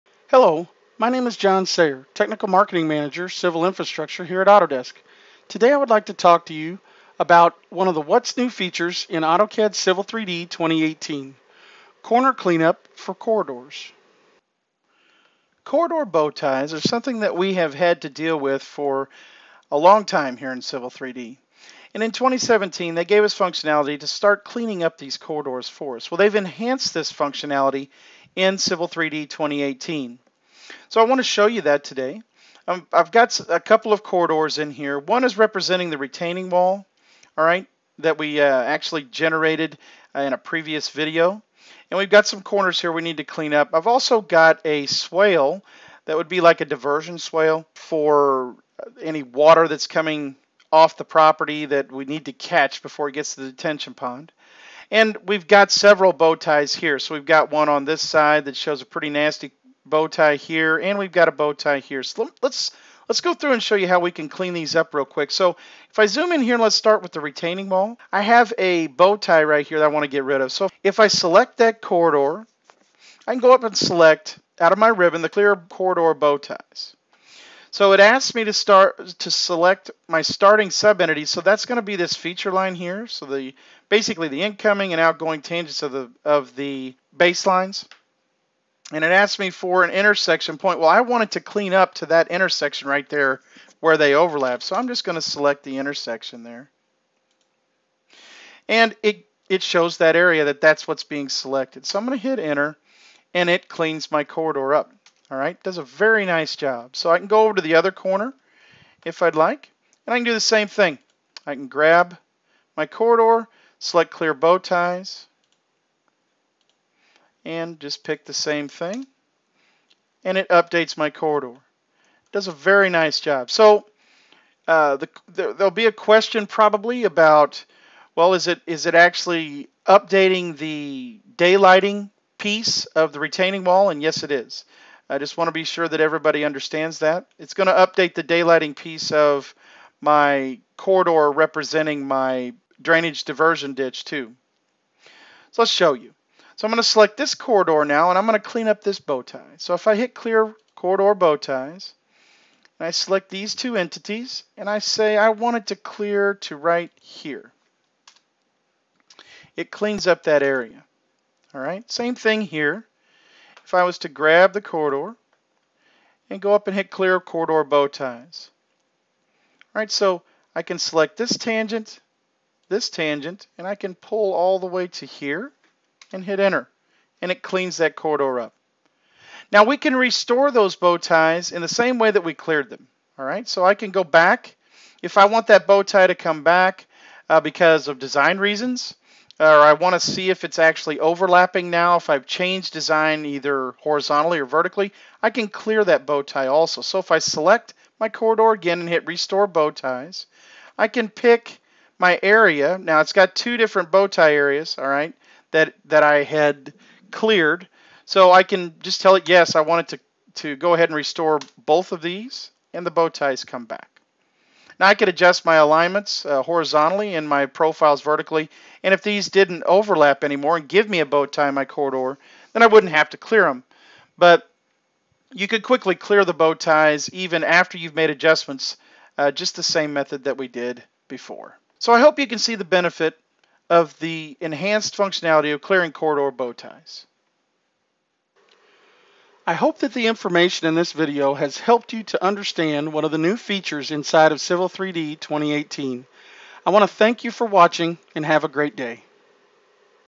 New in Civil 3D 2018: Corner Cleanup for Corridors
Here's how to resolve corridor bowties in daylighting conditions.